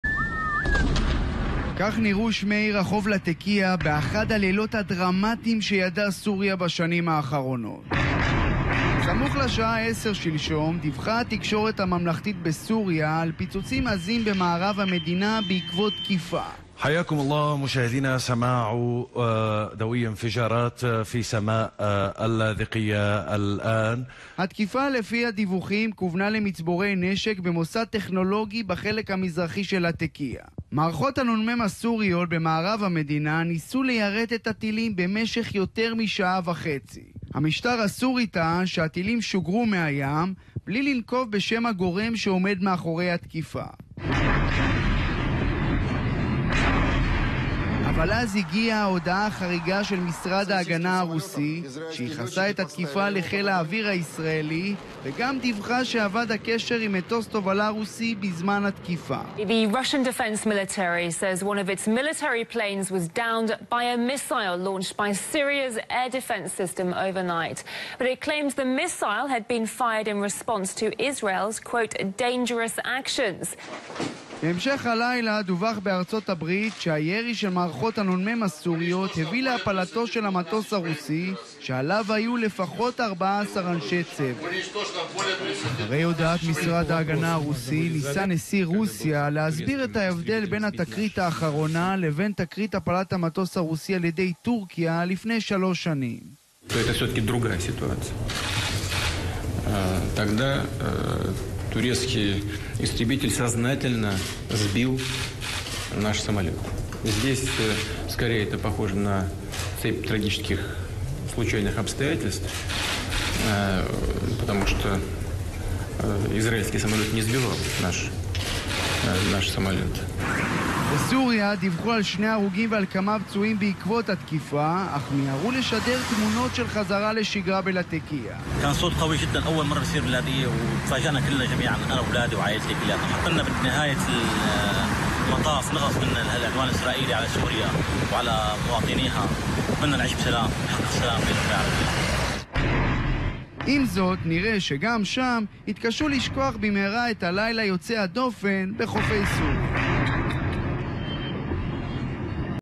לילה לא שקט: ישראל תקפה בסוריה, מערכות נ"מ הפילו מטוס רוסי
התקיפה הישראלית בלטקיה, שכוונה על פי הדיווחים למצבורי נשק במוסד טכנולוגי, קיבלה תפנית לא צפויה: מערכות הנ"מ הסוריות, שניסו ליירט את הטילים במשך יותר משעה וחצי, הפילו מטוס רוסי ובו 15 אנשי צבא רוסים. כתבתו של רועי קייס, מתוך חדשות הערב 19.9.18